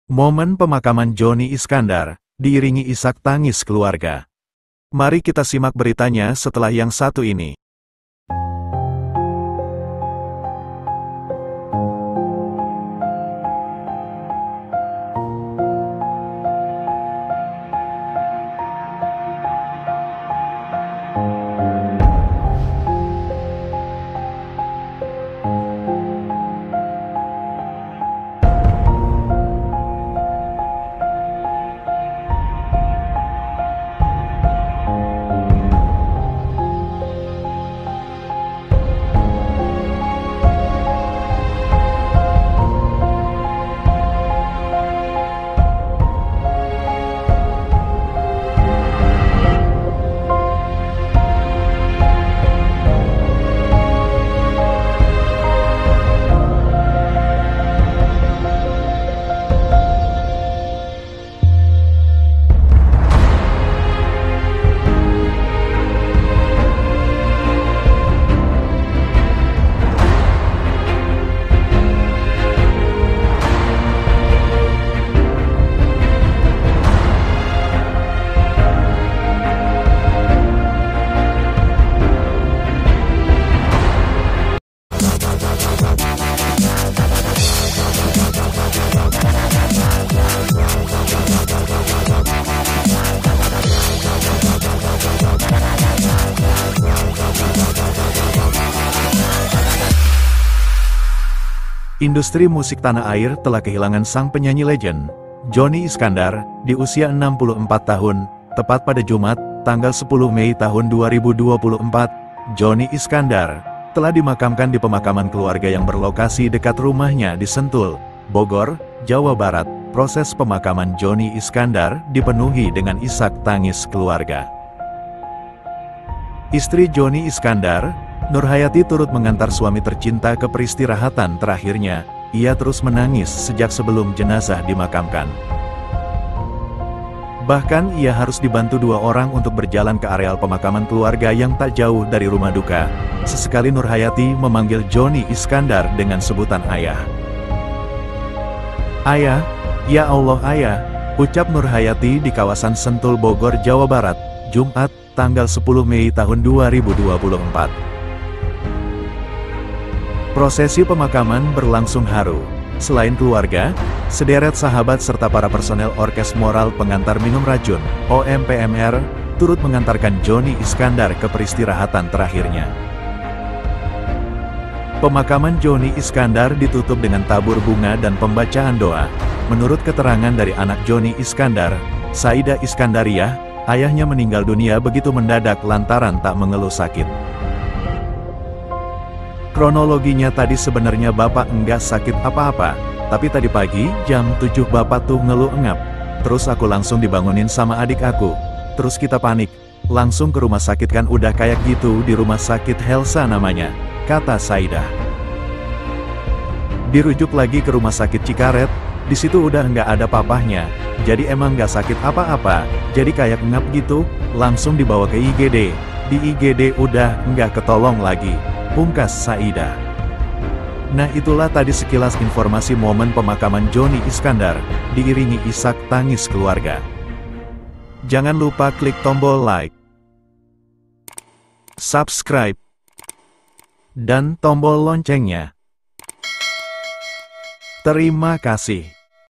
Momen Pemakaman Jhonny Iskandar, Diiringi Isak Tangis Keluarga
Industri musik tanah air telah kehilangan sang penyanyi legend, Jhonny Iskandar di usia 64 tahun. Tepat pada Jum'at (10/5/2024) Jhonny Iskandar telah dimakamkan di pemakaman keluarga yang berlokasi dekat rumahnya di Sentul, Bogor, Jawa Barat. Proses pemakaman Jhonny Iskandar dipenuhi dengan isak tangis keluarga.